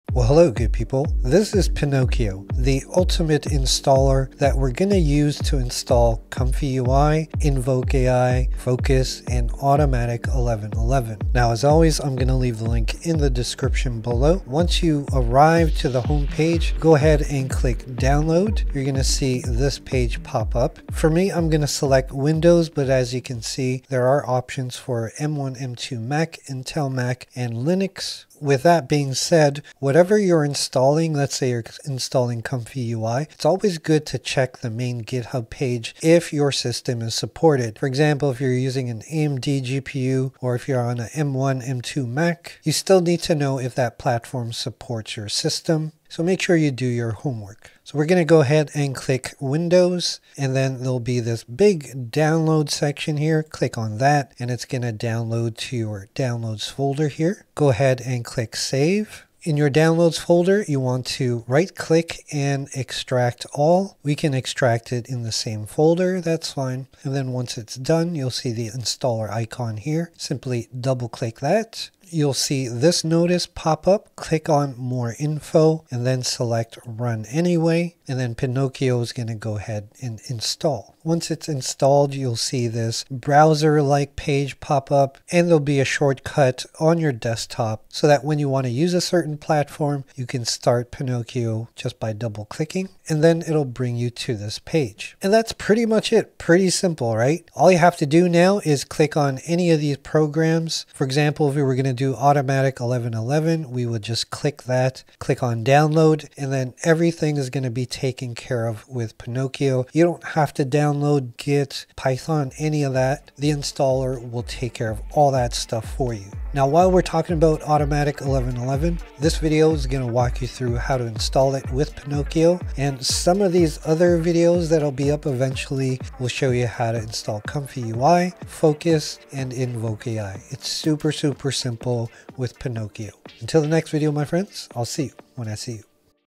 Pinokio The Ultimate Stable Diffusion Installer
Looking for an easy to use and simple stable diffusion installer that can can also install other AI platforms! Look no further than Pinokio! This installer is perfect for beginners and advanced users that want a simple way to install the various stable diffusion platforms and manage them in one place.  I've tried quite a few installers over some time and I have to stay this is one of the best ones I've come across.

⏲Time Stamps
0:00